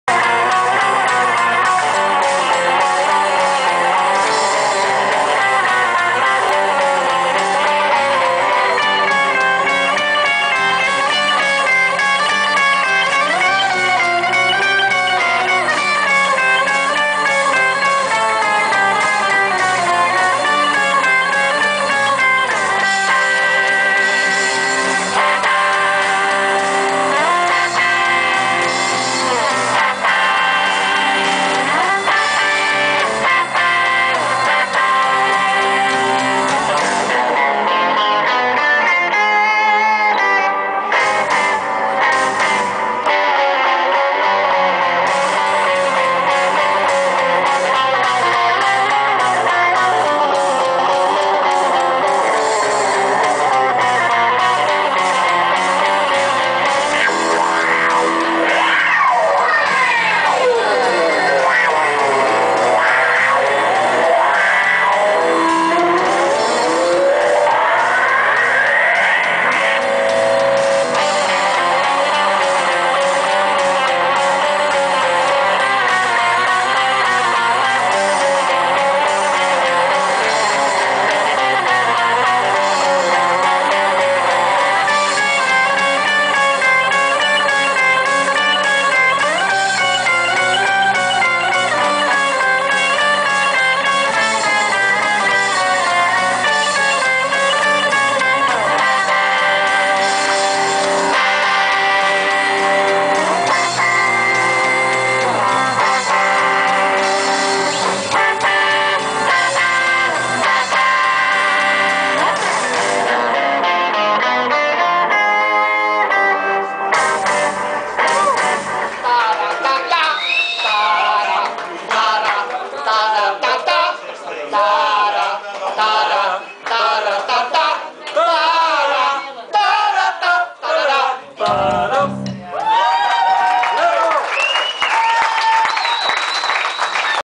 Les Flamands strijken neer in Café Mombasa
LES FLAMANDS (Richard Dielen, Guy en Herman Celis) live in Café Mombasa, Borgerhout op zondag 11 december 2011!